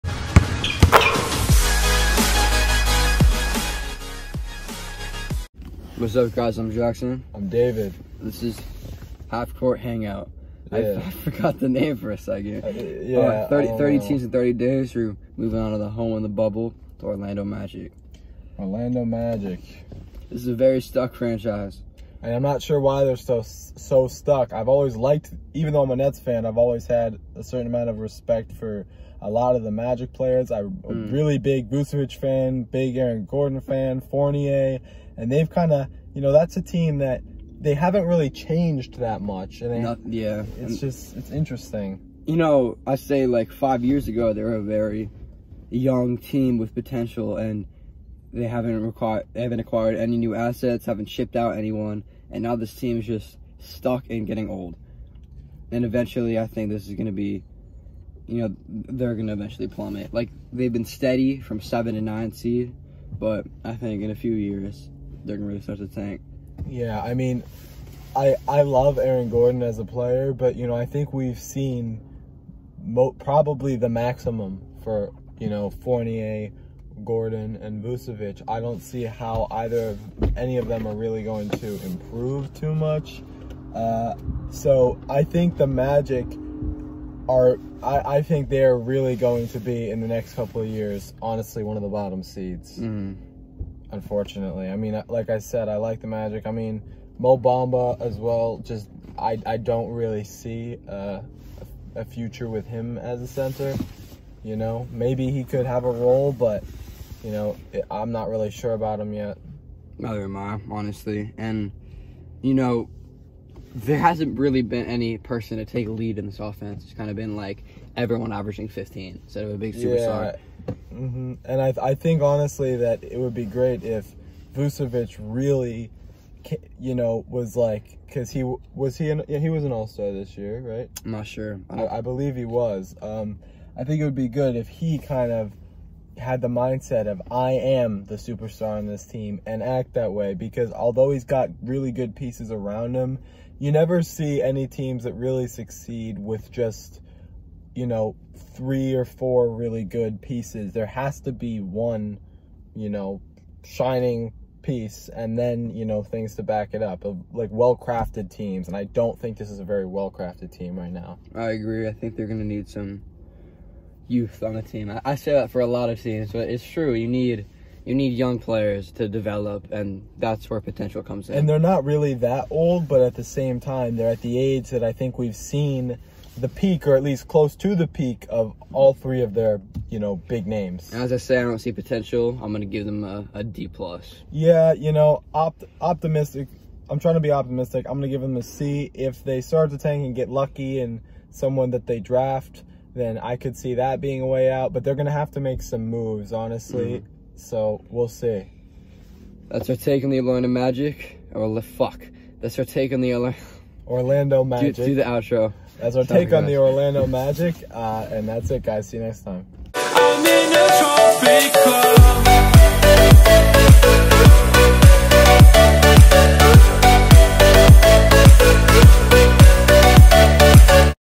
NBA Breakdown: Ep. 24 - Off-season for the Orlando Magic
Welcome to our new series 30 teams in 30 days. In this series, we will be discussing all 30 nba teams going into this offseason, and grading their potential for this season, and seasons to come.
PLEASE NOTE:
These videos are pre-recorded, so we apologize for non updated facts, rosters, etc.